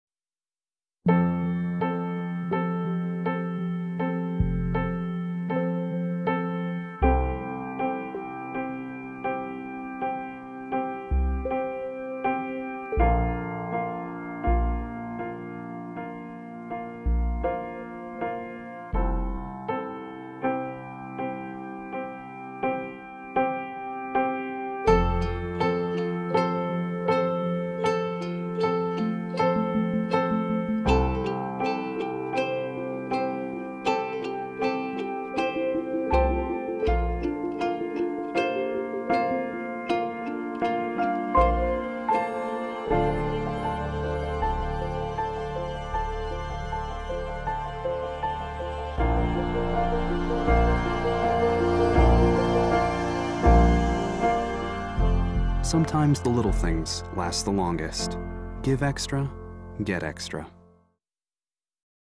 Origami - małe gesty, wielkie znaczenie
Reklama gumy "Extra" przypomina, że dla dzieci, nieważne jest to, jak wielkie rzeczy robimy dla nich jako ojcowie, ale to, jak często jesteśmy przy nich, kiedy nas potrzebują.